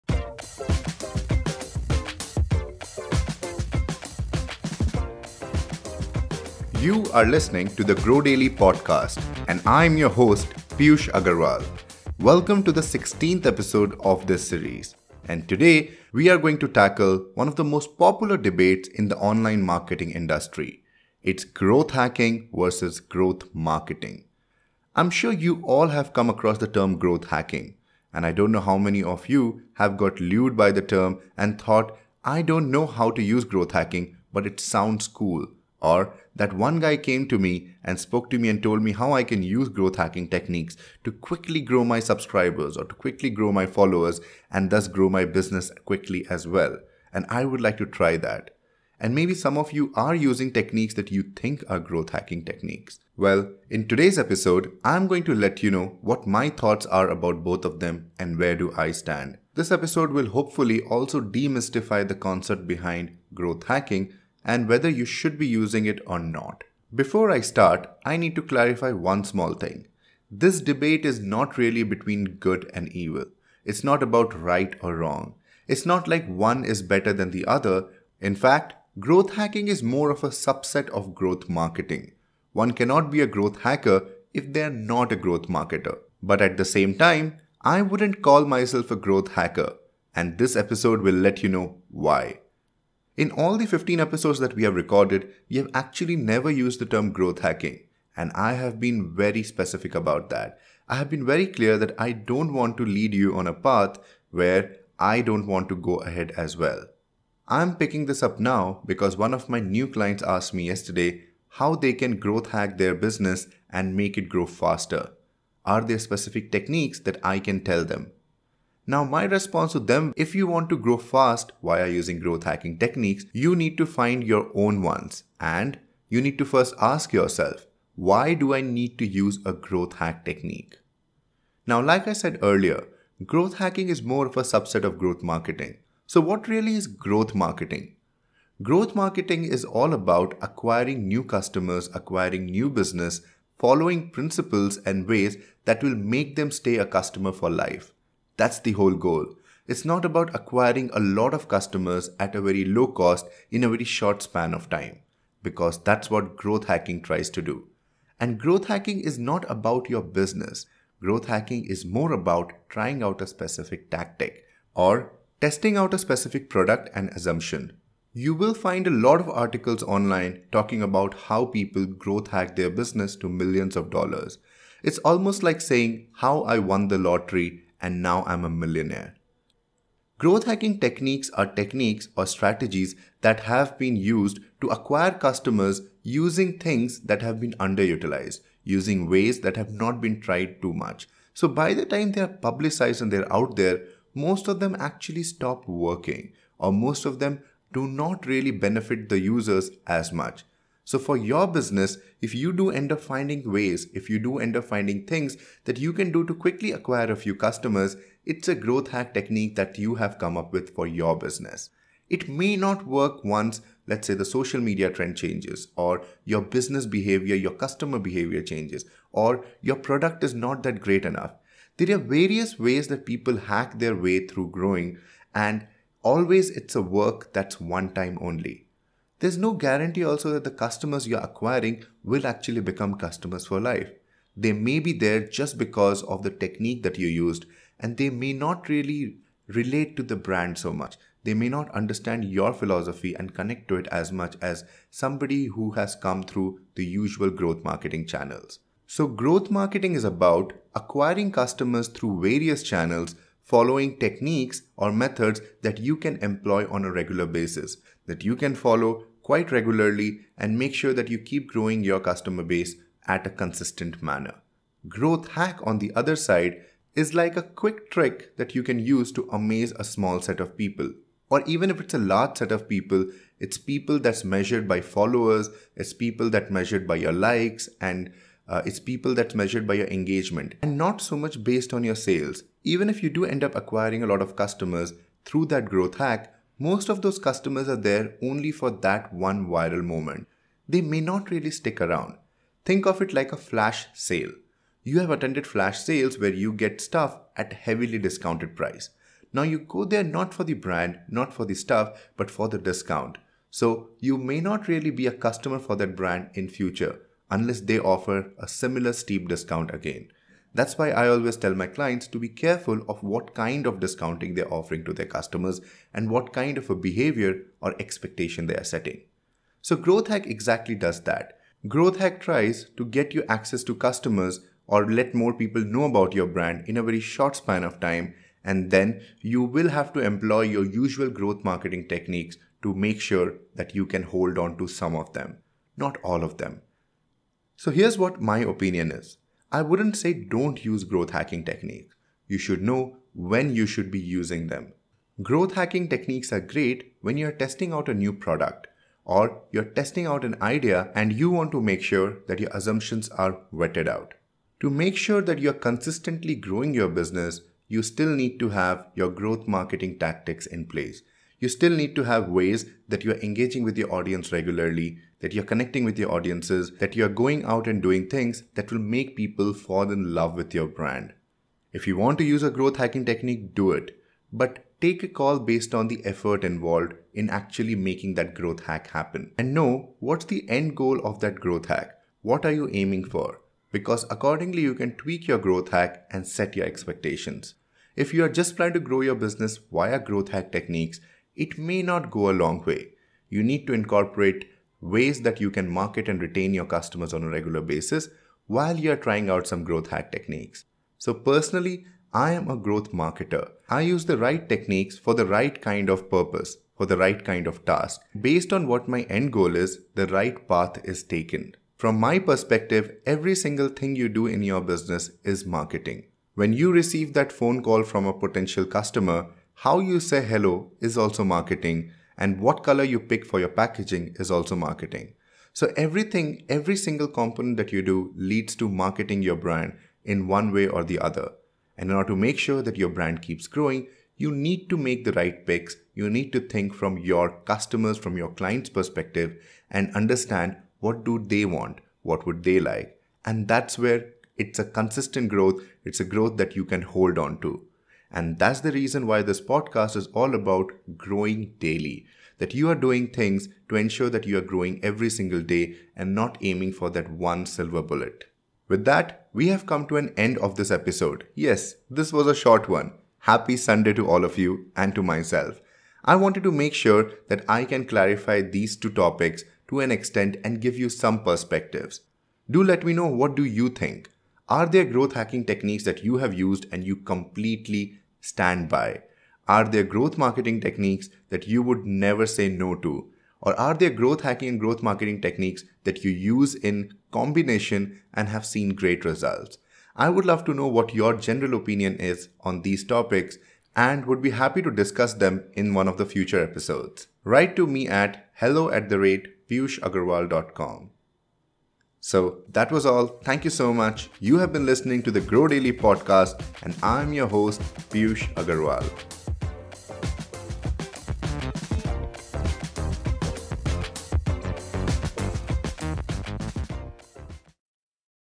Growth Hacking vs Growth Marketing | Grow Daily Marketing Podcast | Ep 16
"Growth Hacking" has become the buzzword in the online marketing space. A lot of people are claiming to be Growth Hackers who can help grow your business in a very short time. Piyush uncovers what growth hacking is all about and what is the right way to leverage it.

In this episode he covers the following:

- What is Growth Hacking and why is it different that Growth marketing?
- When should you use Growth Hacking?